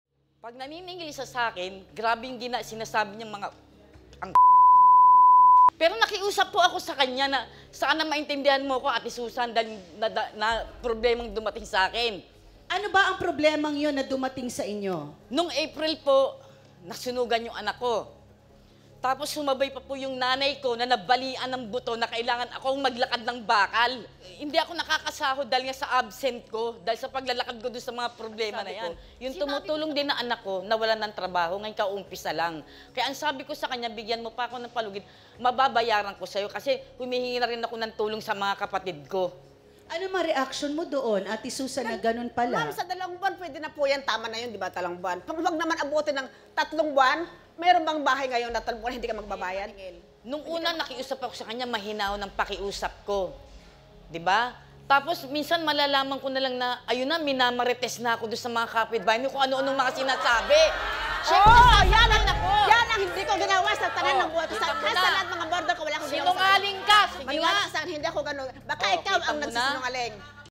Upa mo, bayaran mo!
Tama bang maging mahigpit sa paniningil ng upa?